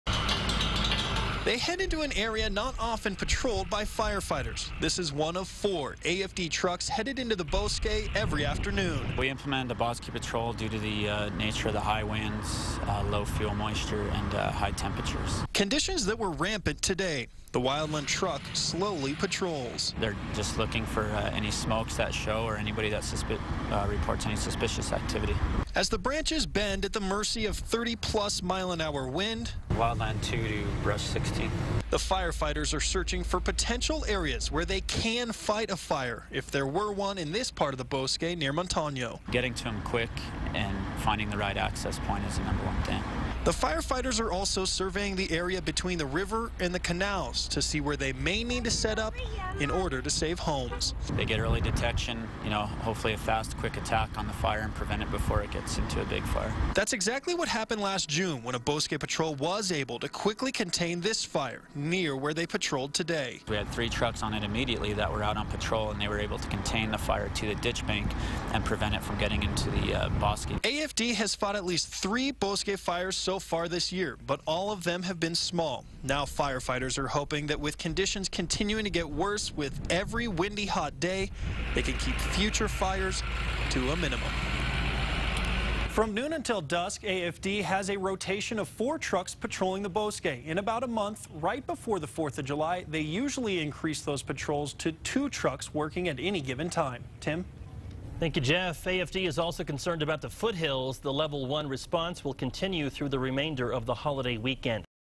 Firefighters monitor bosque for threats
Temperatures creeping above 90 degrees and winds blowing more than 30 miles per hour on Sunday are causing Albuquerque firefighters into the bosque to prevent devastating fires.